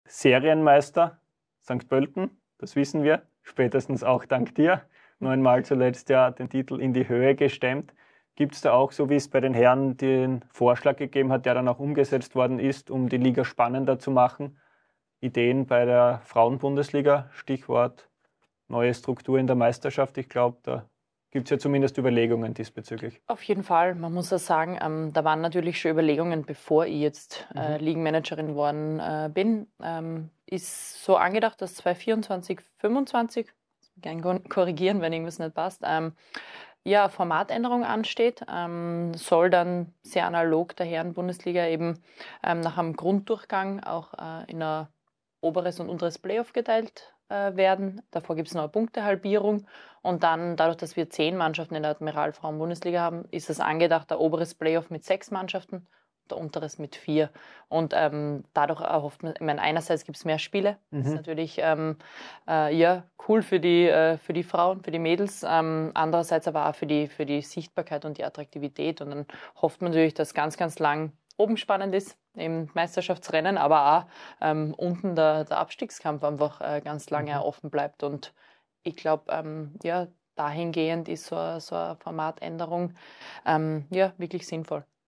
Zukunft der ADMIRAL Frauen Bundesliga
Carina Wenninger erklärt uns, wie sich die ADMIRAL Frauen Bundesliga in der Zukunft weiterentwickeln soll! 💪

Was sagt ihr dazu? Habt ihr Vorschläge? 🤔